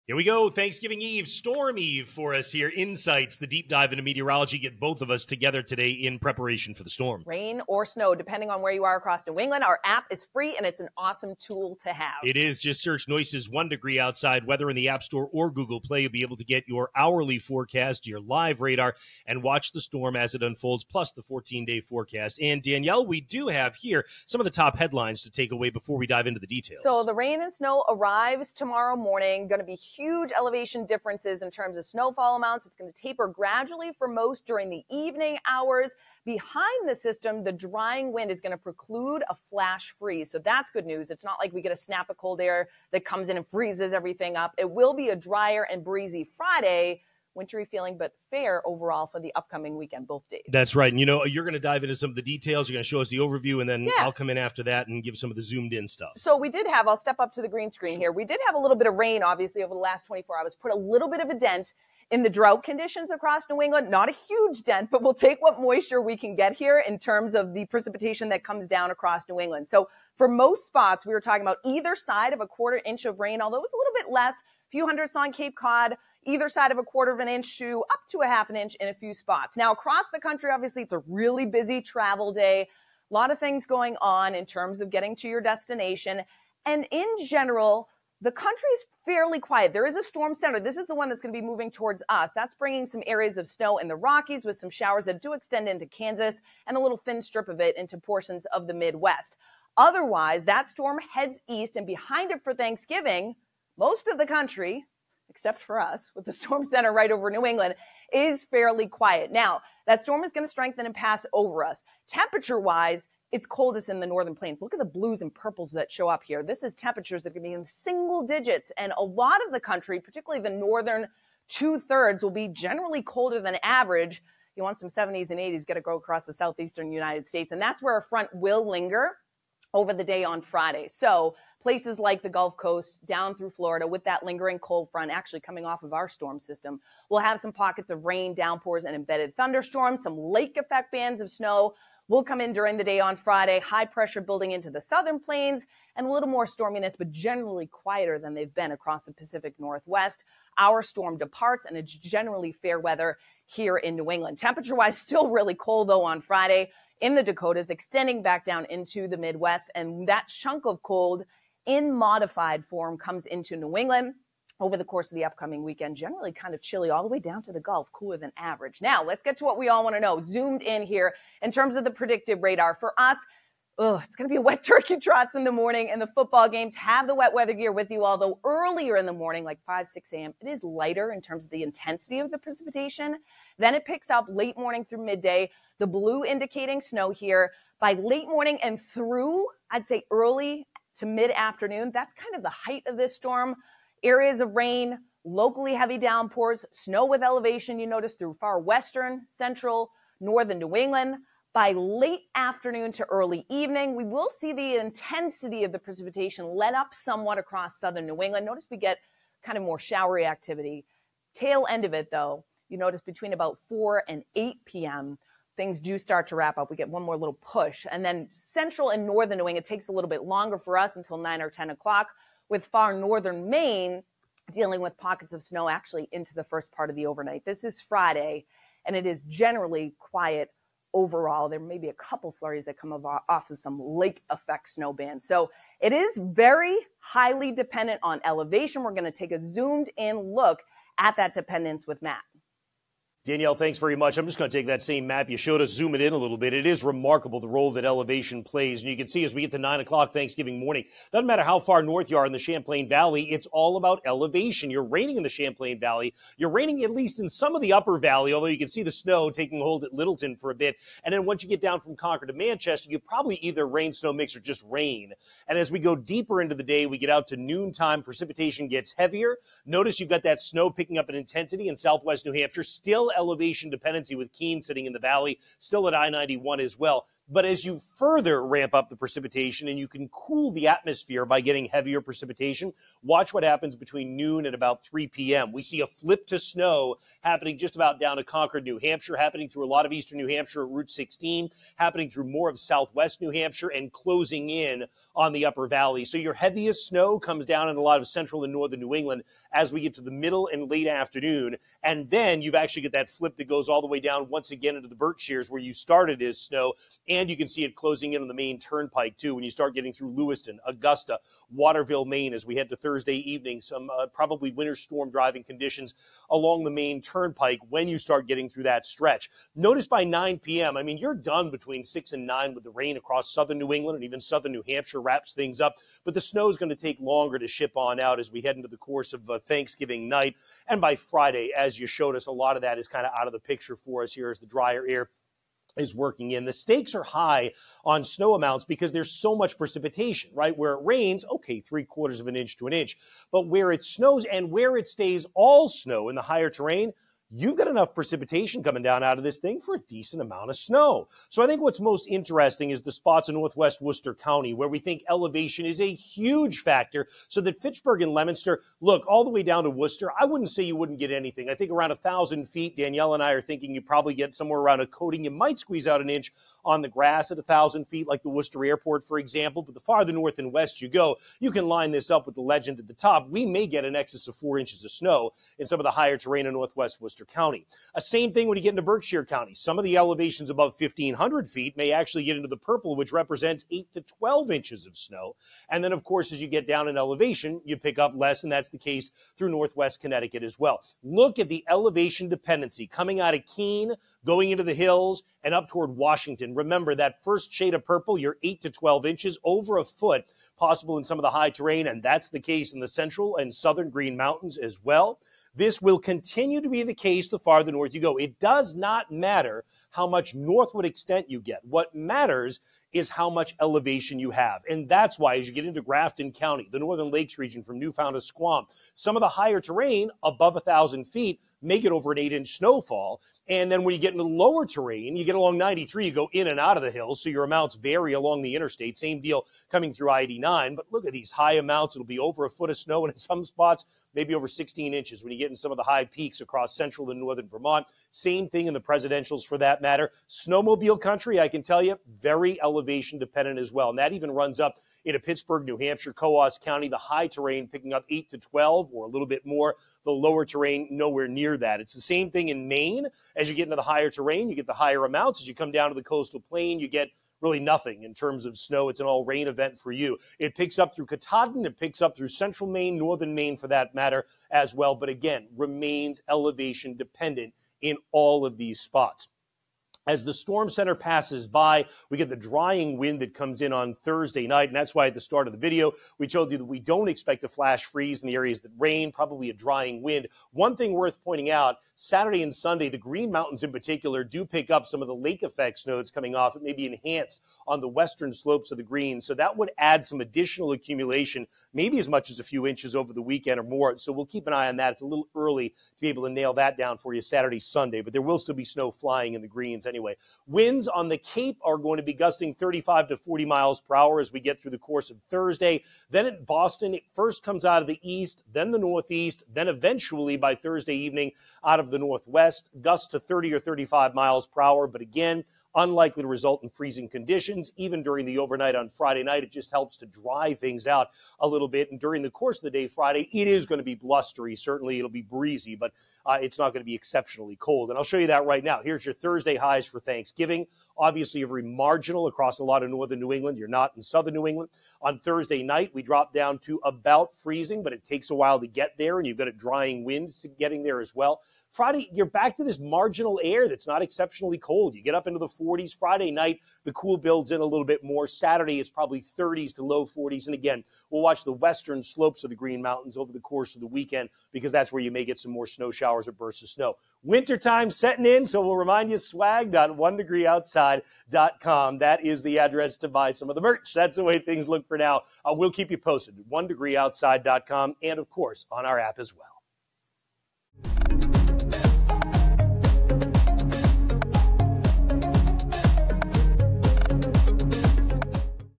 Wednesday Insights - Thanksgiving Storm! Matt & Danielle Team for a Deep-Dive Into Snow, Rain & Wind
Matt Noyes and Danielle Noyes team up to explain the Thanksgiving storm:
* Rain for much of Southern New England.
* Heavy, wet snow in the mountains with sharp elevation contrasts.
* Gusty winds for Cape Cod and Eastern Massachusetts.
* Snow continues in the mountains through Thanksgiving evening.
📲 Download our free Noyes' 1DegreeOutside Weather App for localized storm tracking, live radar, and 14-day forecasts.

Note: This is an updated version of the original video, which wa published about 40 minutes earlier. We saw a contouring error in the snow map (the more intricate they are, the greater the risk for error!) and fixed it, posting this new video as quickly as possible. Thanks! -M&D